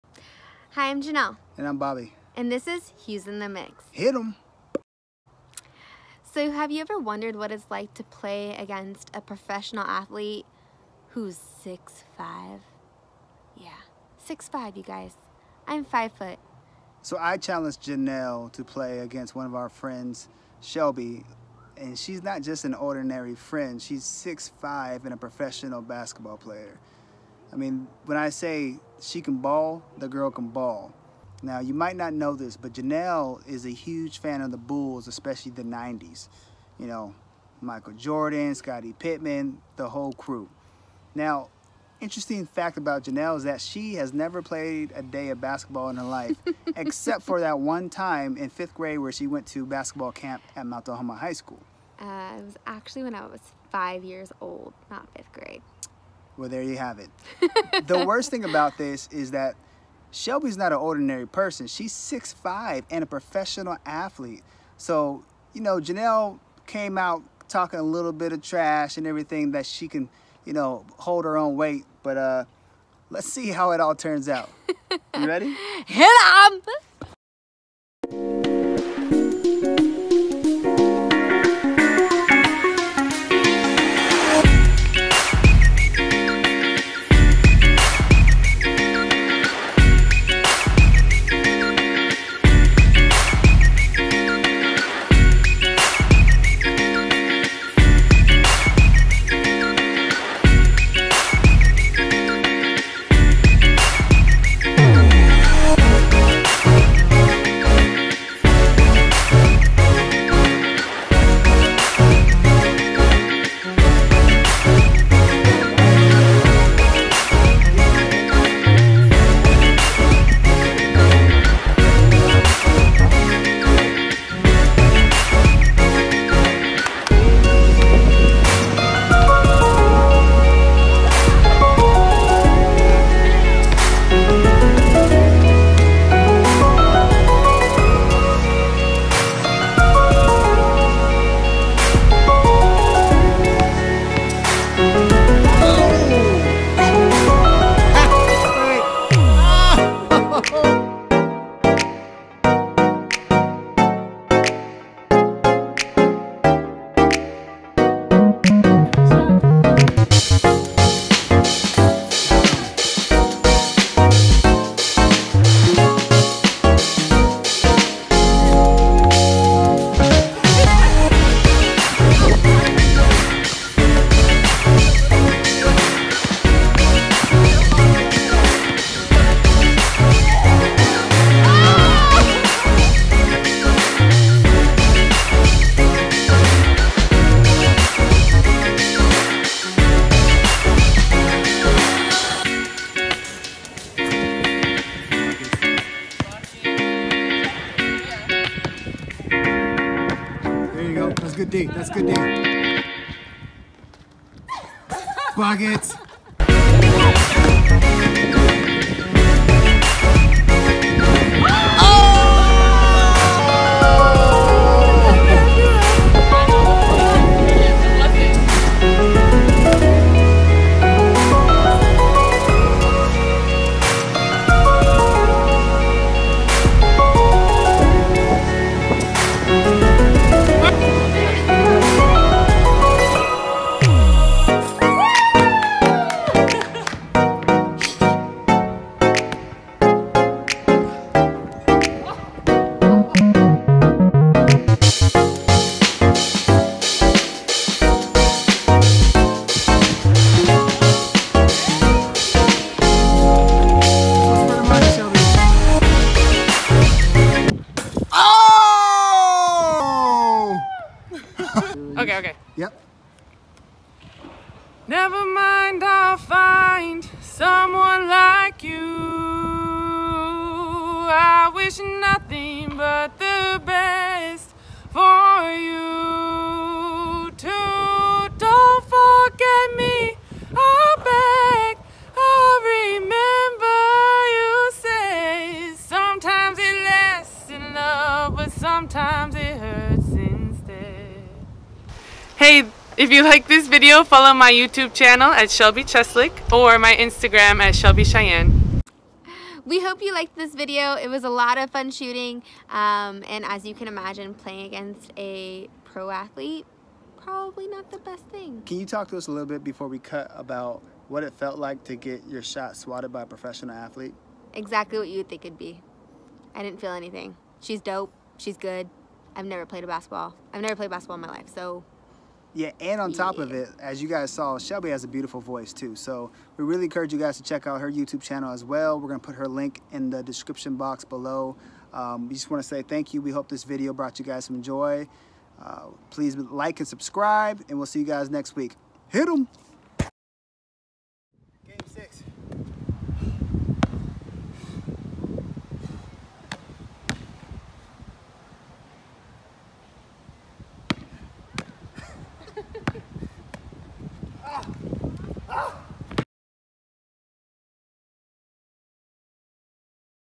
6’5” VS 5’
I had a great time with my friends Bobby and Jenell playing a little one on one. Not everyday you get challenged by your 5 foot tall friend that has never played before. I had so much fun shooting this video with them. Hope you enjoy :)